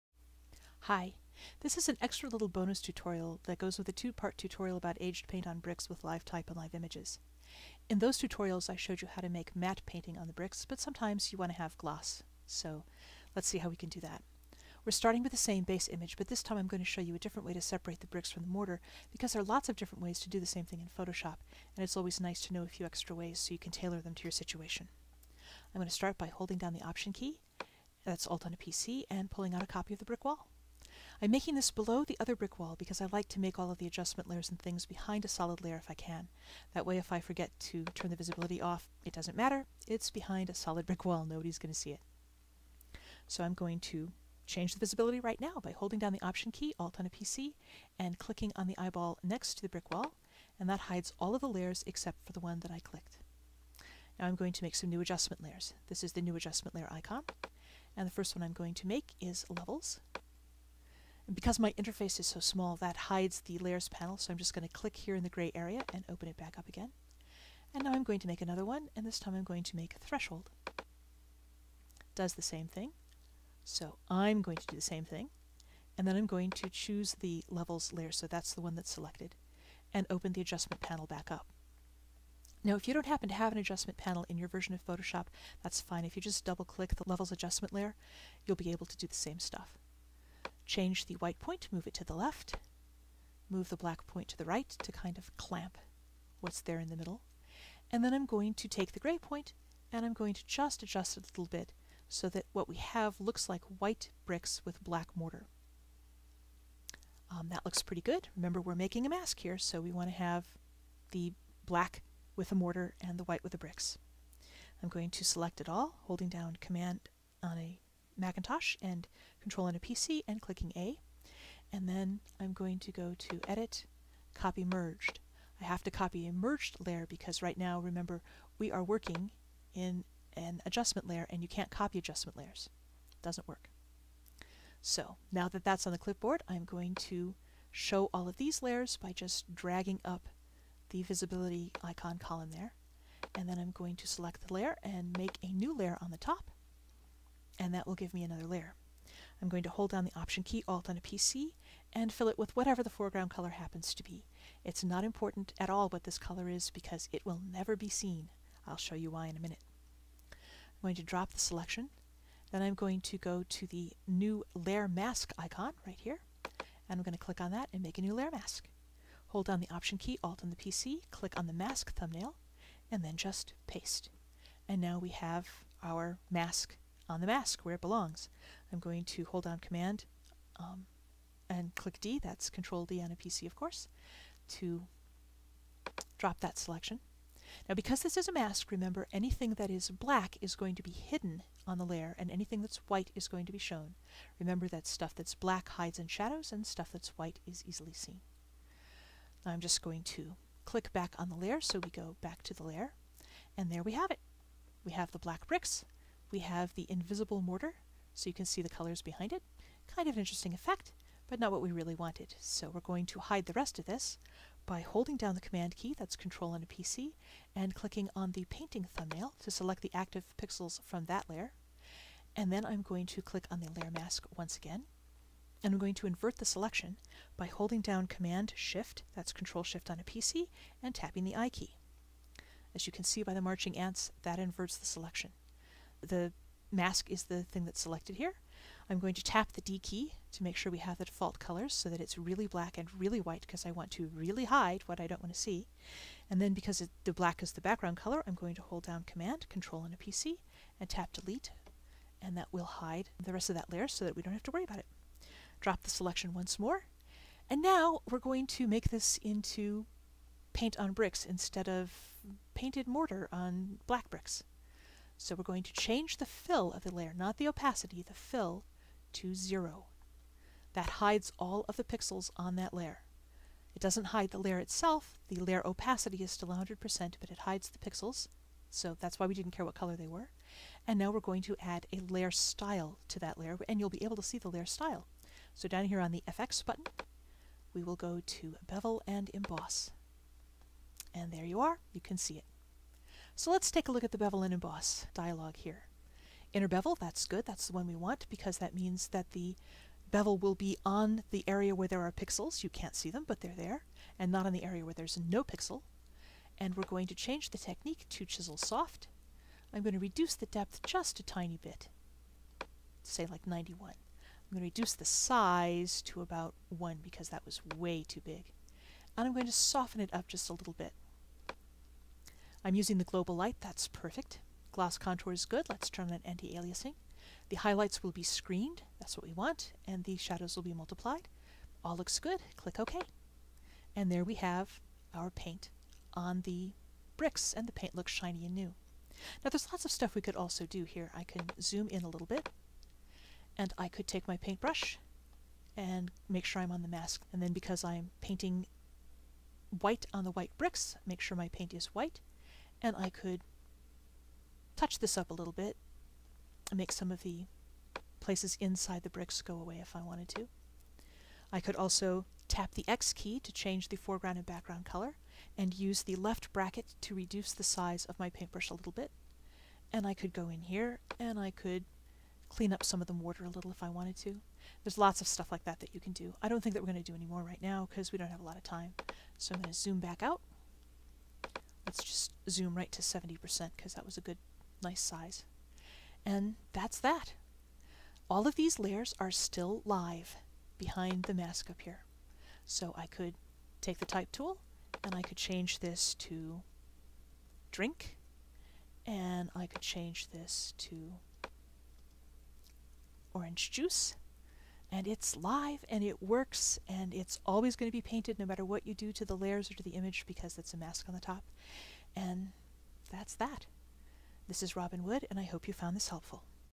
Live Paint on Brick - Gloss - Photoshop Tutorial
Bonus segment to go with the other two Live Paint on Brick tutorials. This one is a stand alone; another way to make the mask, and an end result that shows shiny new paint.